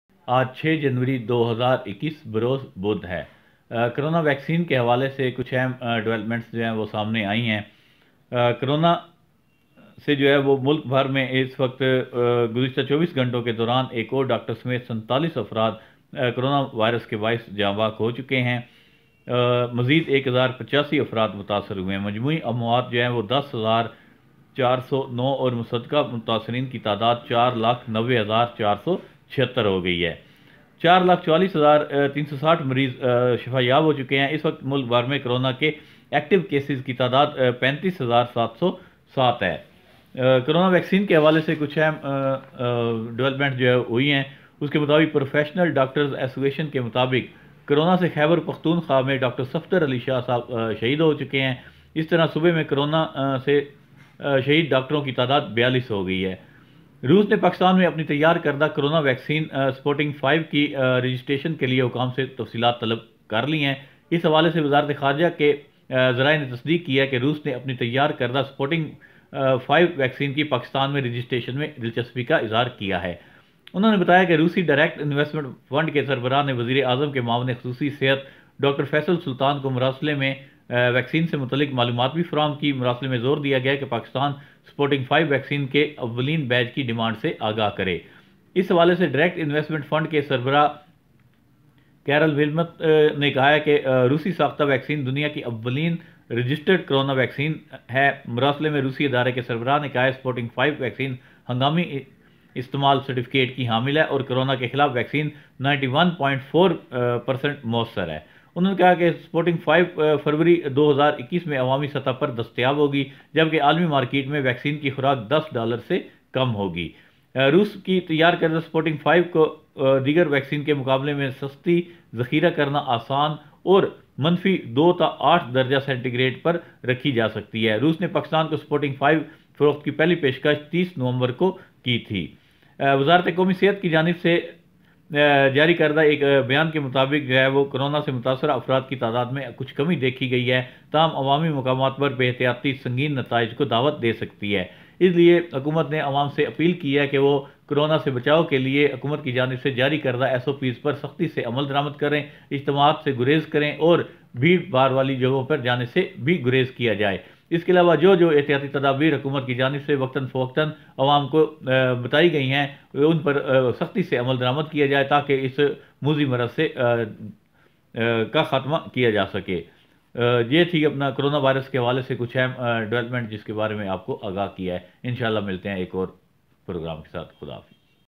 corona virus ki vaccine pakistan may kab dastayab ho gi hakoomat nay bata dia
corona virus k dunia bhar may war jari hain aysay may kai mumalik nay is ki vaccine ki tiari ka dawa kia ha jin may china , Russia,or degar mumalik shamil hain pakistan ko china and Russia nay is ki vaccine ki offer ki ha jis par pakistan govt. nay ikdamat shuru karnay ka aylan kar dia ha